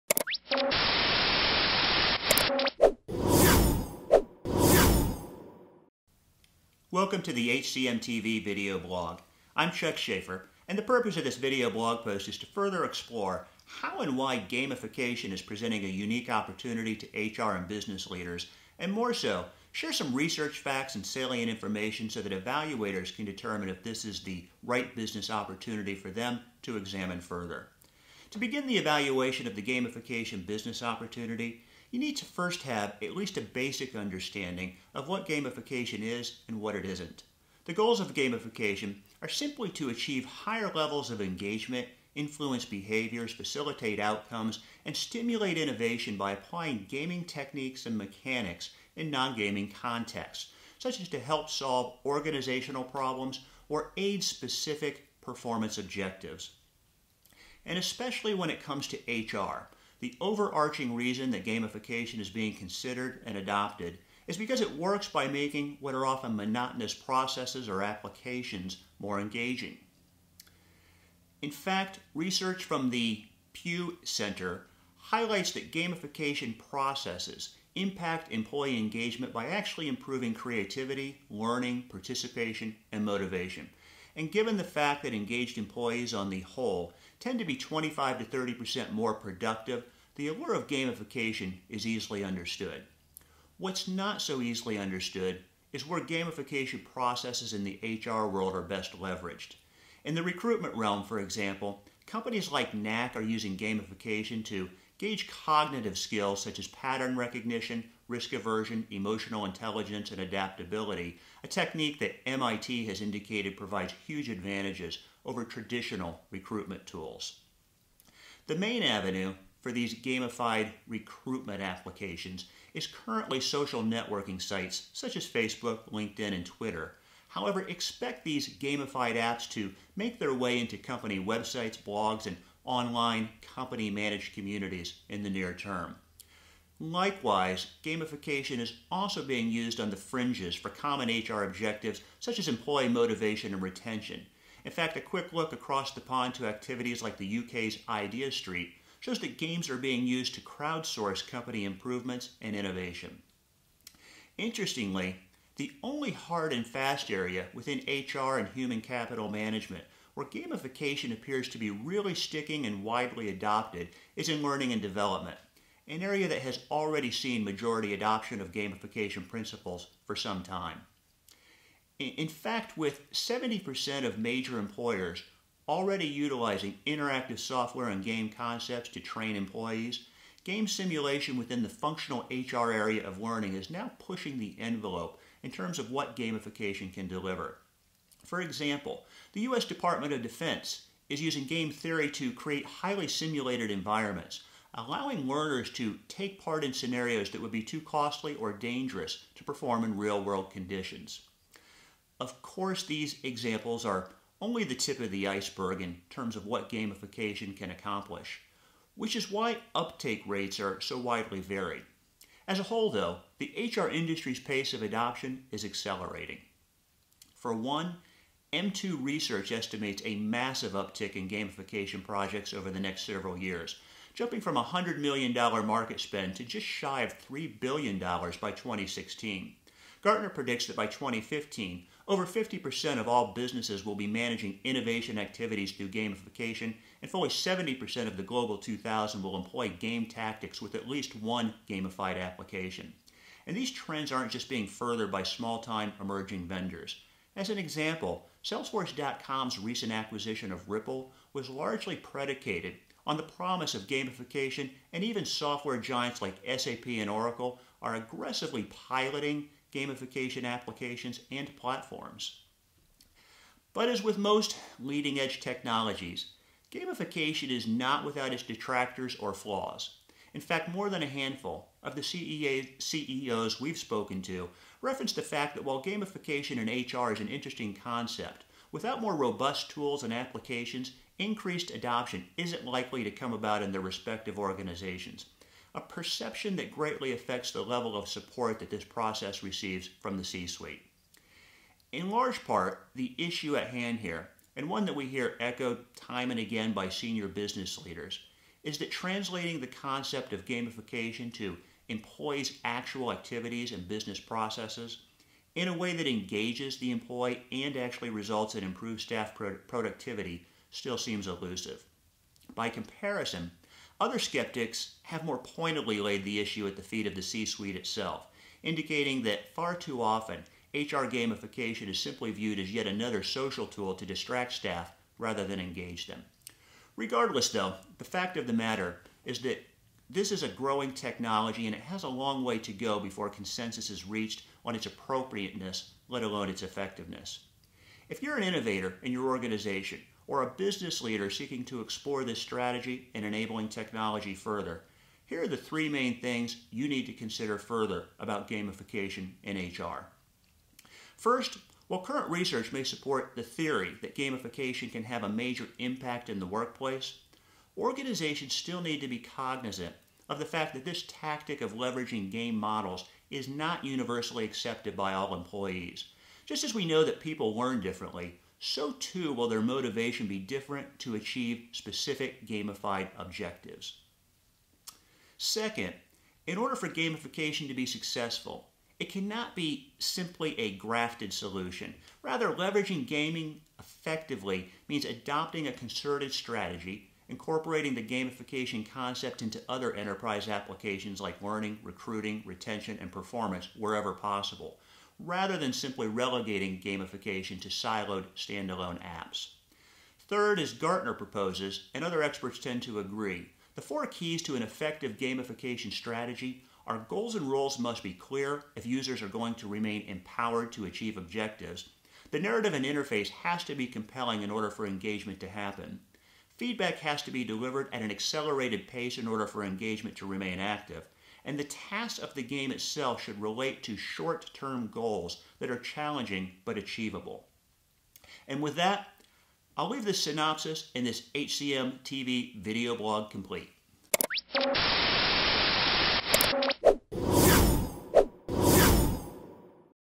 Gamification applied to HR
In this HCM.tv video blog post, Chuck examines how and why gamification offers a unique opportunity to HR and business leaders to improve employee engagement, influence behaviors, facilitate outcomes and stimulate innovation by applying gaming concepts and techniques to non-gaming contexts. He also shares gamification research facts, some examples which have delivered big paybacks and recommendations in how to get started from analysts such as Gartner. This HR video blog post empowers HR and HCM leaders with the research points and salient information so they can determine if gamification is the right business opportunity for them to examine further. Additional HCM and HR gamification articles are available and related HCM, HR and payroll reviews, videos, podcasts and rich media content are available